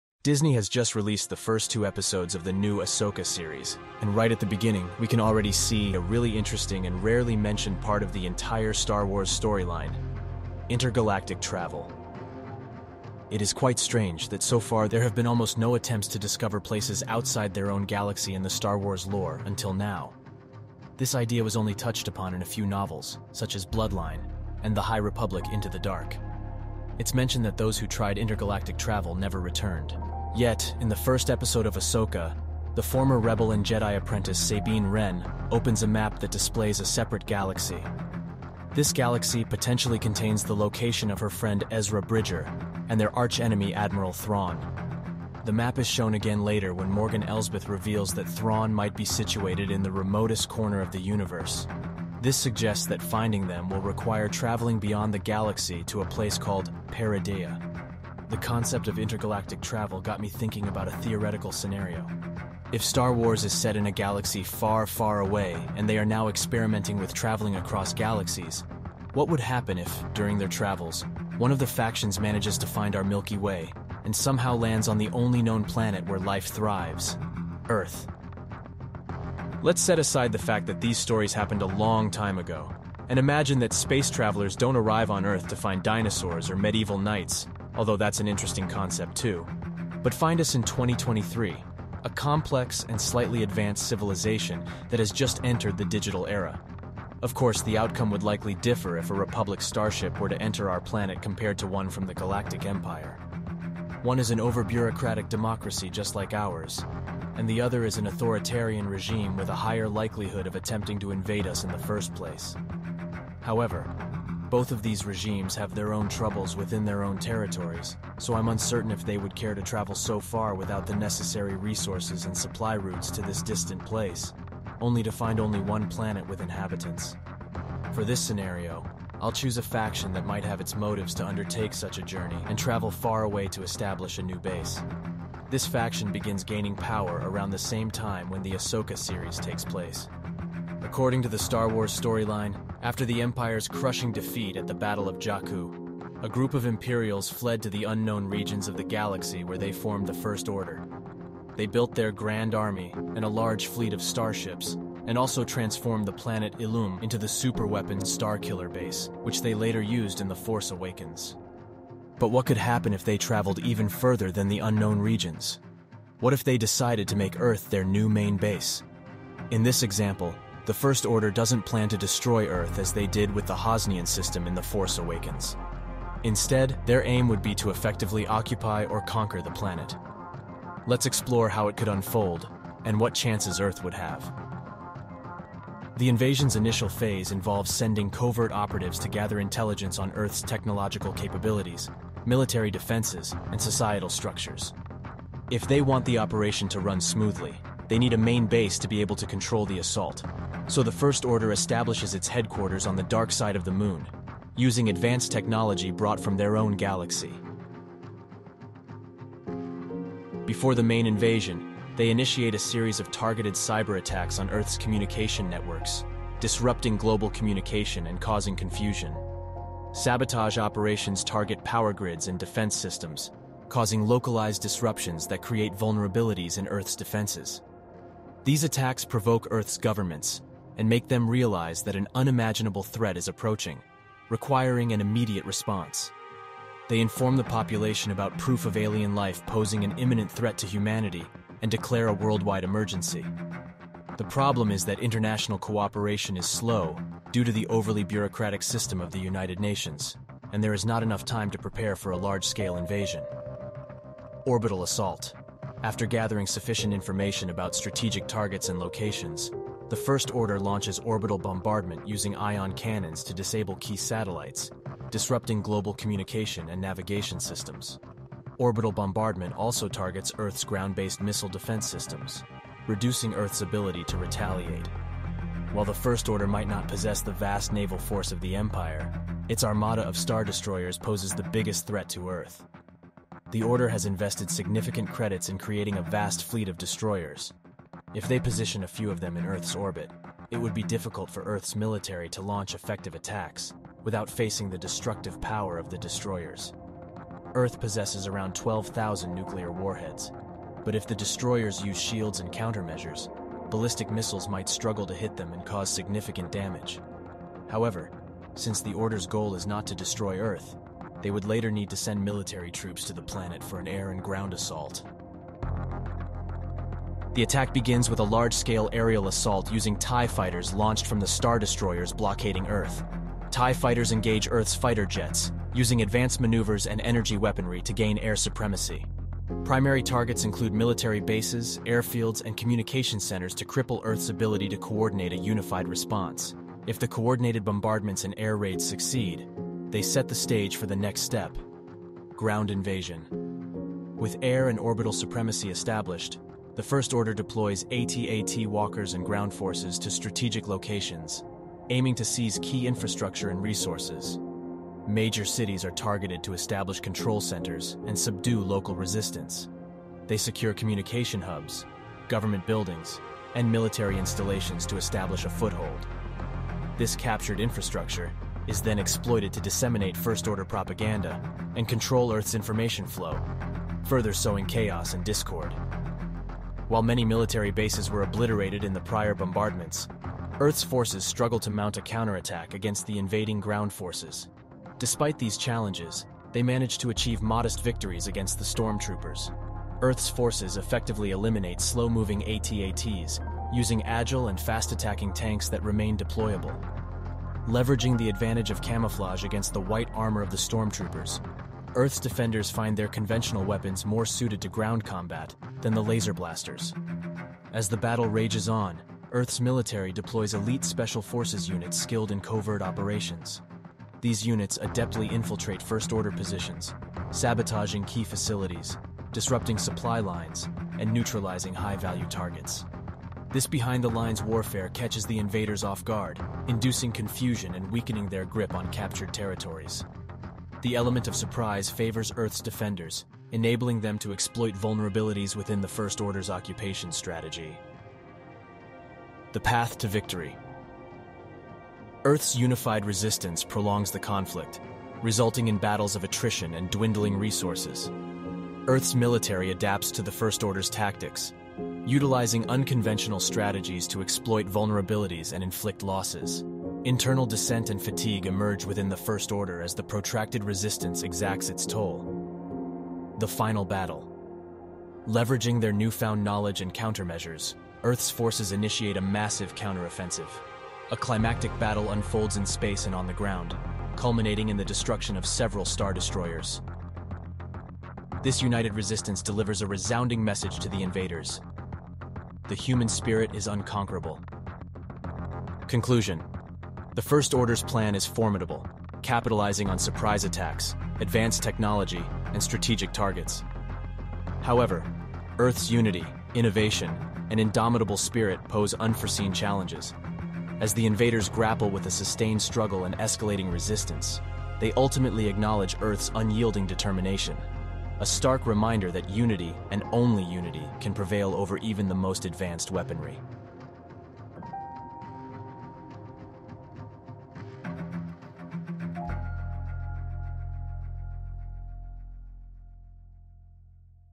What If Stormtroopers Invaded Earth? AHSOKA Theory Unveiled
What Happens If A Military Power Like The First Order Manages To Reach Our Earth And Start A Full Scale Invasion?

I had a lot of trouble this time, working with AI Tools, for example Midjourney has a really hard time blending images of modern warfare and fictional. I believe they also have some ethical code not to make content about actual conflicts (Chatgpt told me, that it won't make a midjourney prompt for this :D )

Maybe I will create some more scenarios on my own, in the future...

I just watched the first two episodes of Disney's Ahsoka Series, and saw a good opportunity to create this theory about intergalactic travel, and the invasion of. Earth.